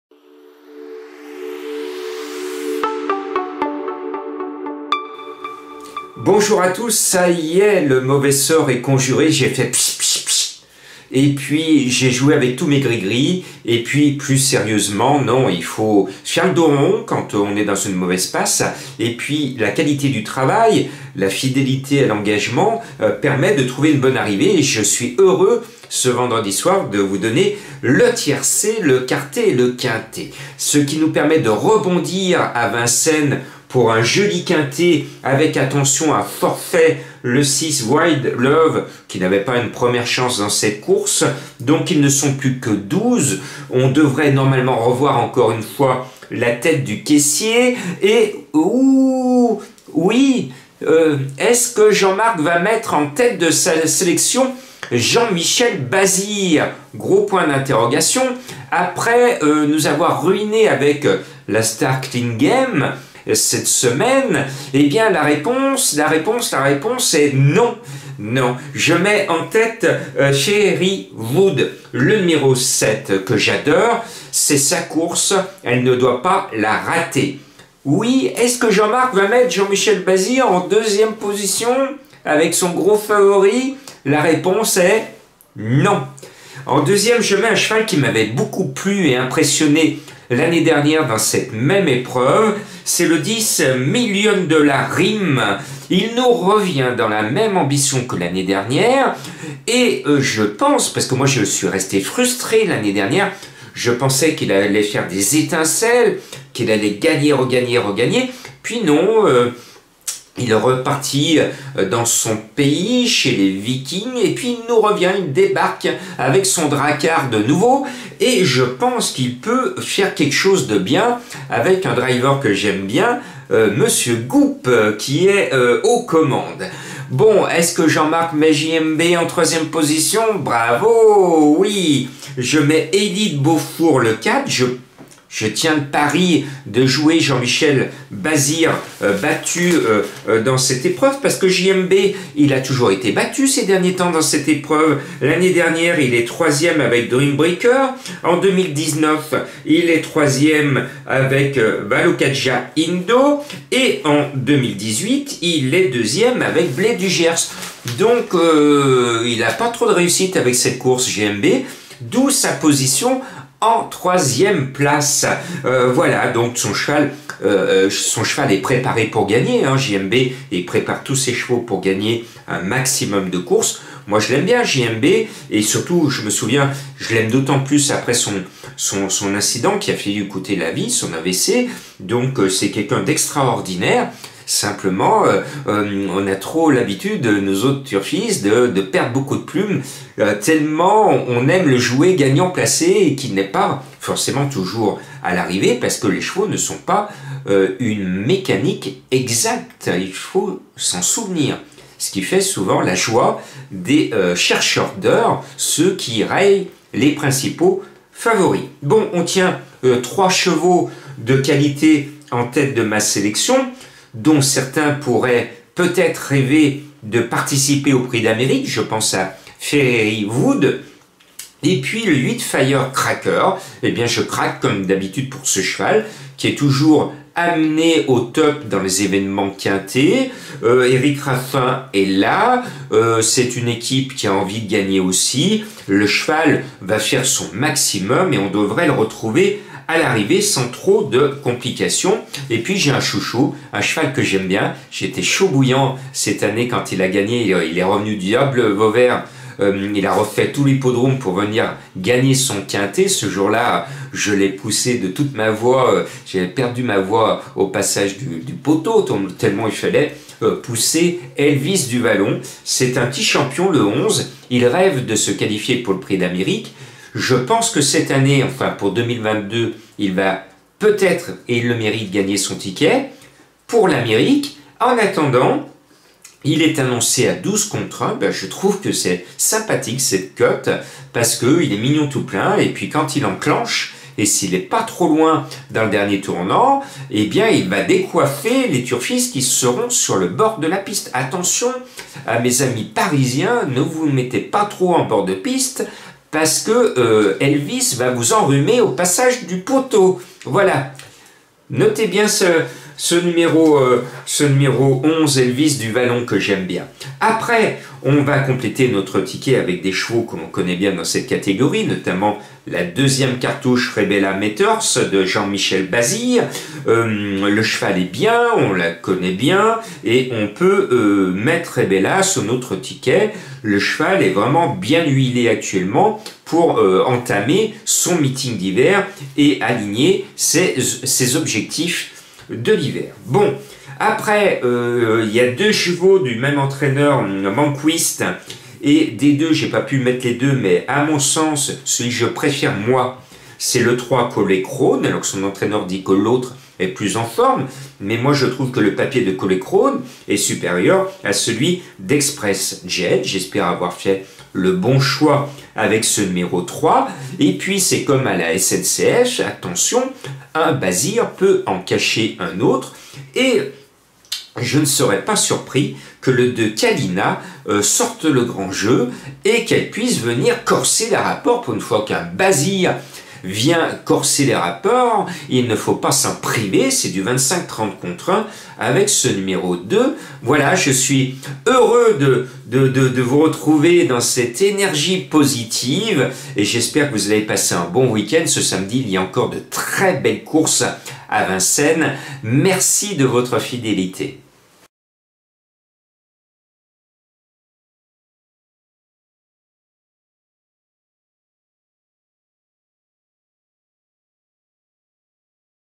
pronostic pmu quinté+ du jour samedi 13 novembre 2021 Vincennes réunion 1 course 3
sélection Jean-Marc Roffat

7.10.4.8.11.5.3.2

Confiance 5

le 6 non partant